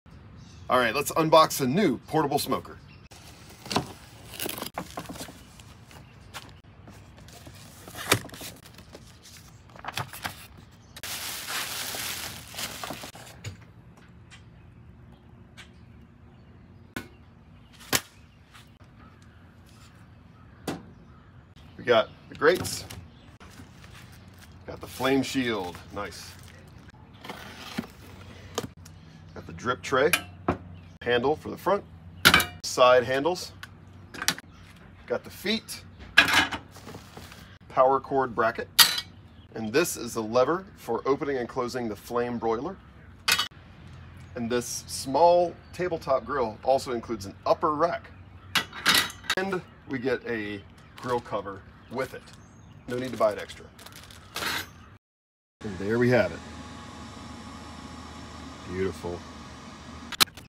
Pit Boss Pellet Grill Unboxing / Unboxing the Pit Boss Tabletop Pellet Grill
pitbossnation pitboss We got a new toy! The Pit Boss tabletop portable pellet grill. Check out the unboxing and initial burn-off!